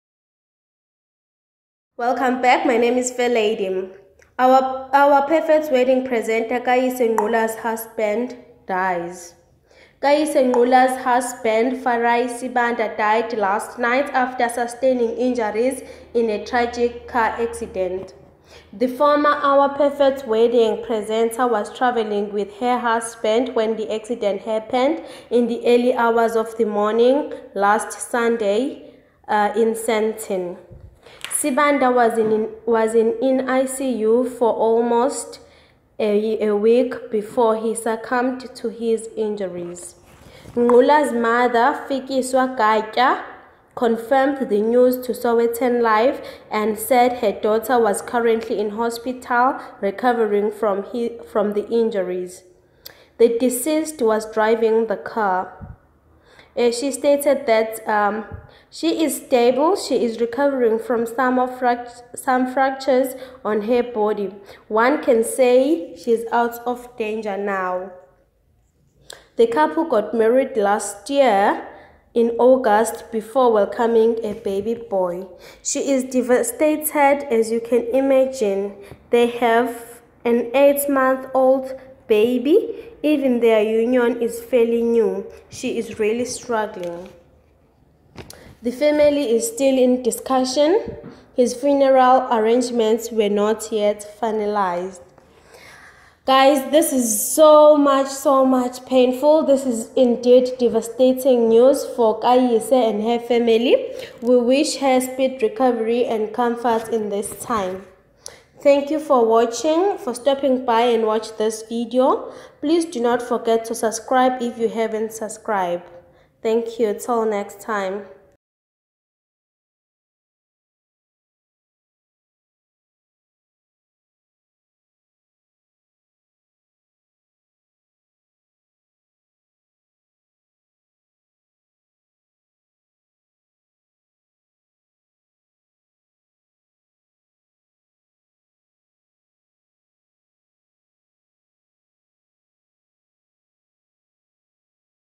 Former OPW presenter Kayise Ngqula loses hubby in car crash
Former Our Perfect Wedding presenter Kayise Ngqula loses hubby in car crash